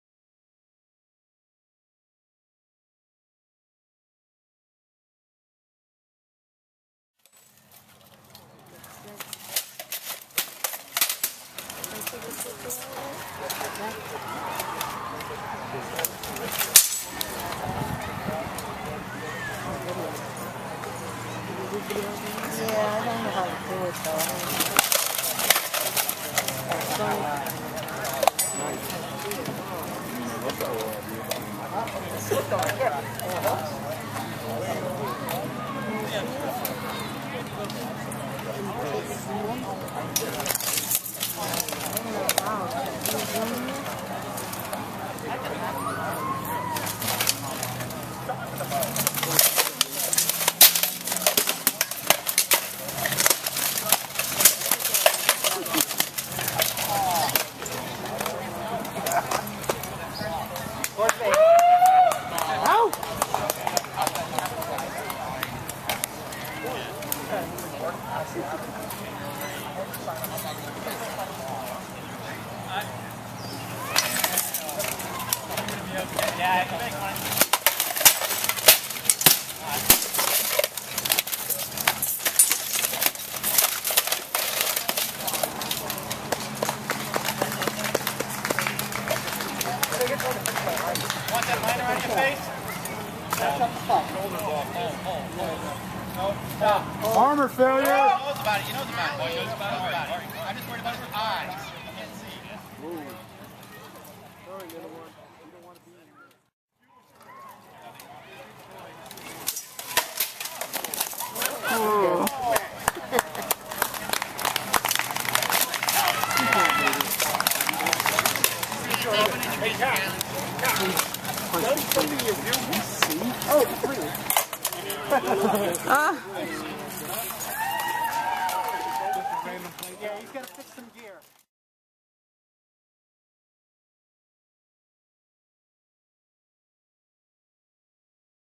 More dueling scenes from the 3rd annual Medieval Faire, Johnston, RI, 5/18/2013
If you like duels, knights in shining armor, tales of olde, the place to be on Saturday, May 18, was the 3rd annual Medieval Faire, held at Johnston's Memorial Park. Miss today's events? You're in luck, as this year, it is a two-day event, so visit the park on Sunday, May 19 for more Medieval fun, music, crafts, belly dancing performances, and duels.